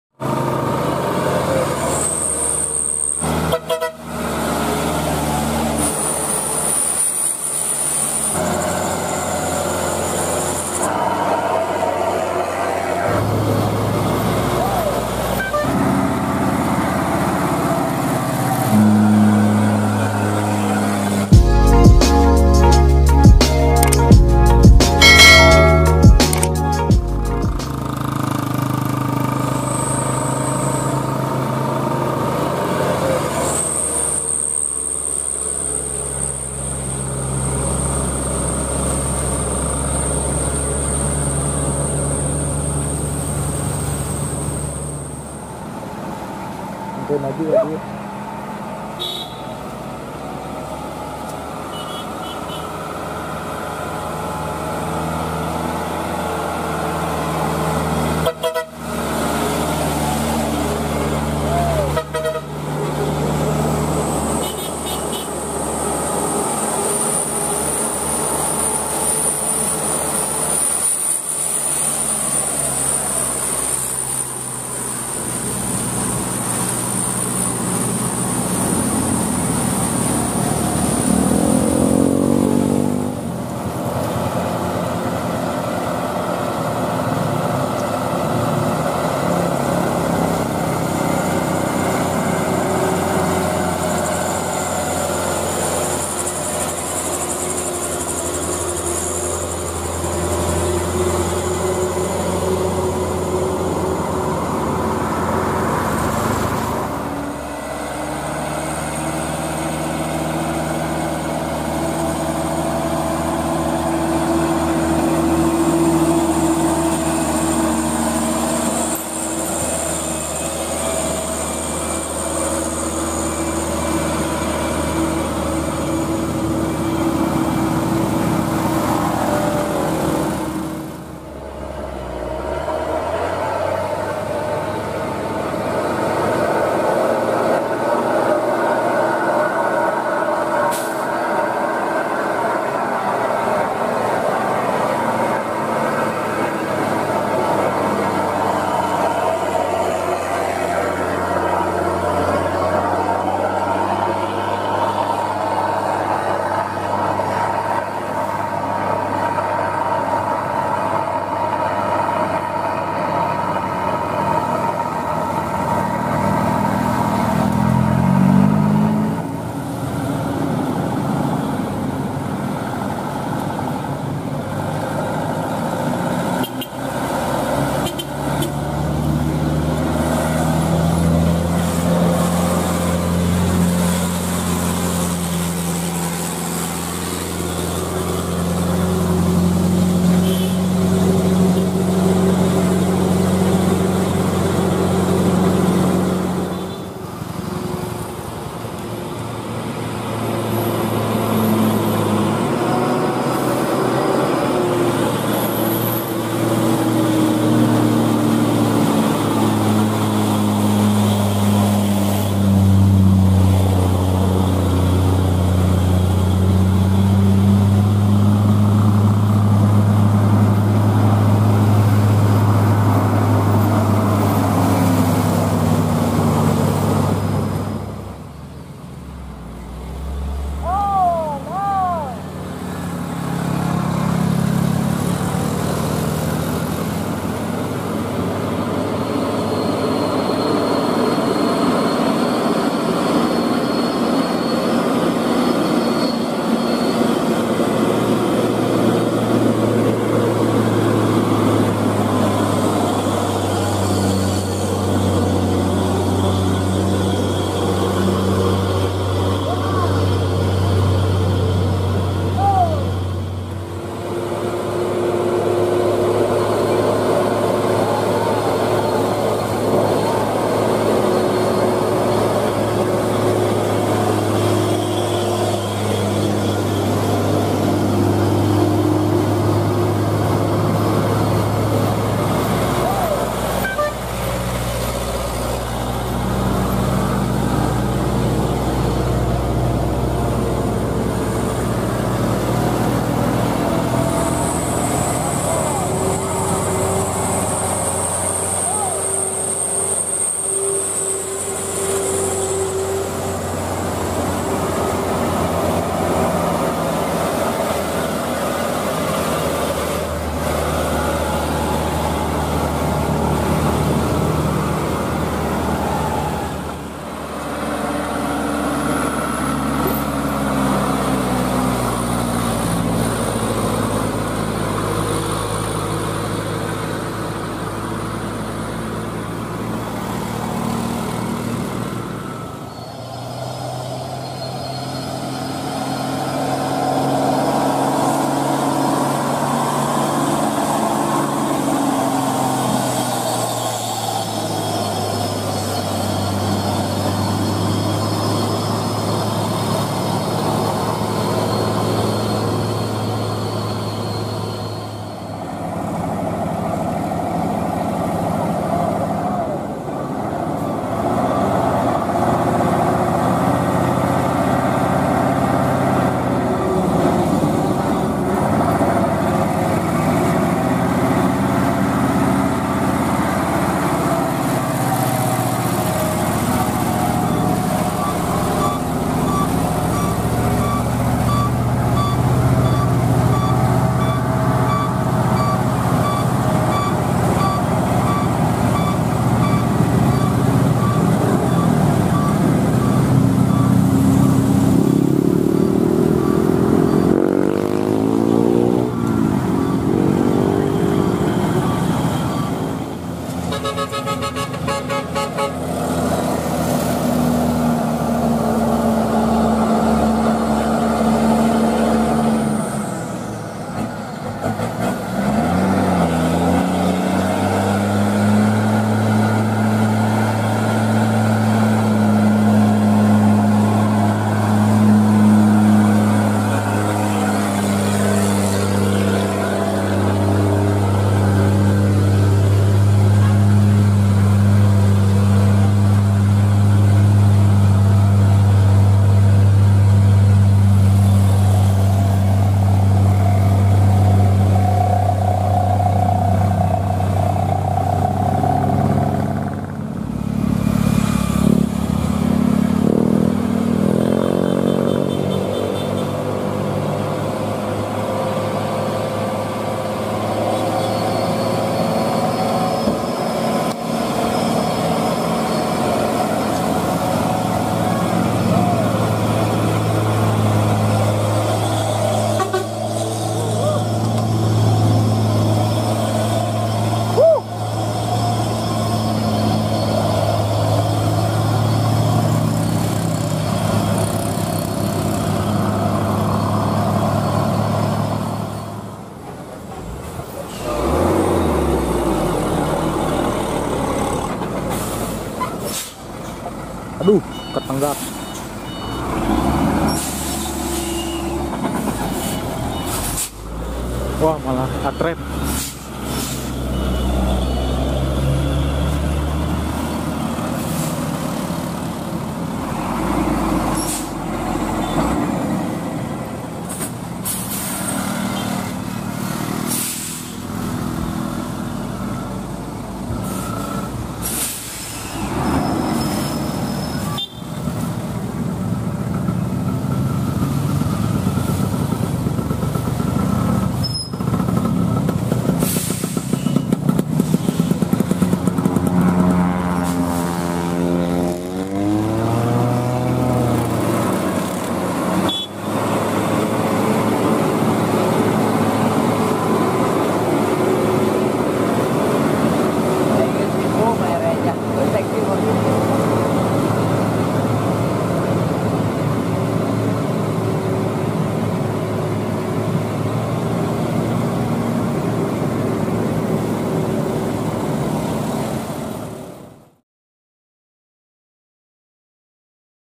Adu Kekuatan Dump Truk Nissan VS Hino, Dump Truk Muatan Pasir Dan Batu Di Tanjakan Tepung Kanjut
Lokasi Ditanjakan Tepung Kanjut Kota Banjar Jawa Barat